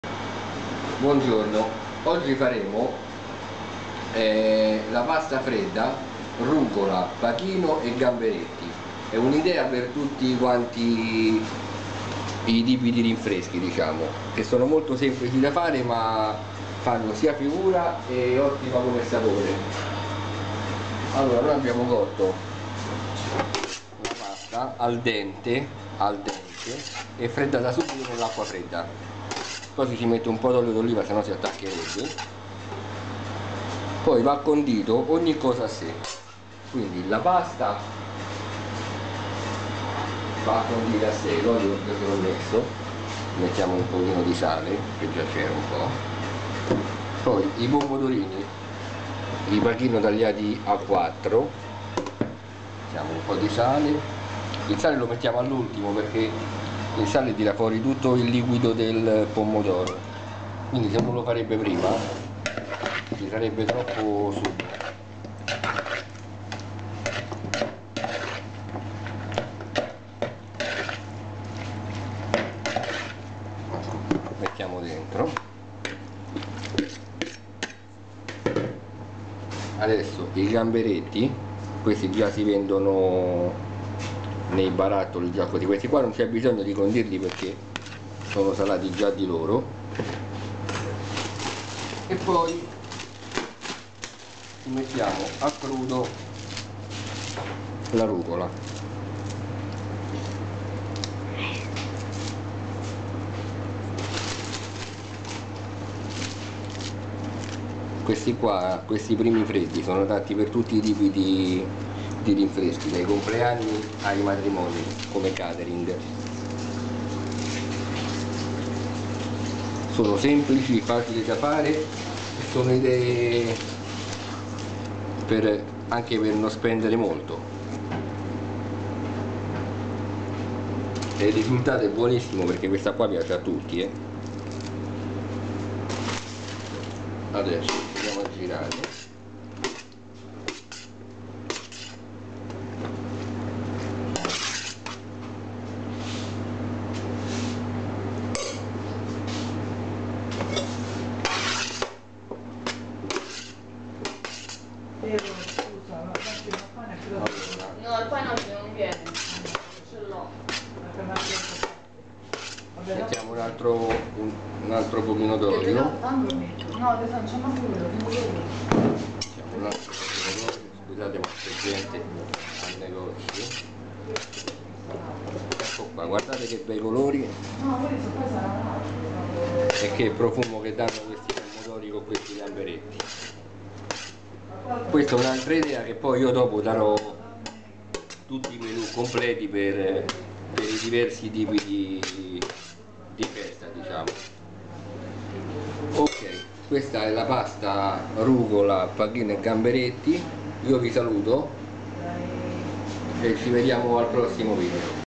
Pasta Fredda Gamberetti Pachino e Rucola : Ideale per Rinfreschi : Ricette Dolci e Cucina
In questo video spiego come fare la pasta fredda gamberetti pachino e rucola,ideale per tutti i tipi di rinfreschi Ricette dolci e cucina